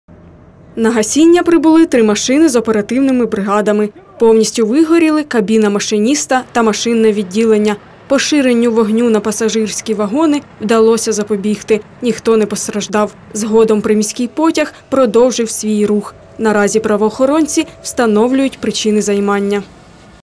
На Сумщині загорівся локомотив...
На Сумщині загорівся локомотив пасажирського приміського потягу. Пожежу помітили близько 9-ї ранку на станції Біловоди в Роменському районі.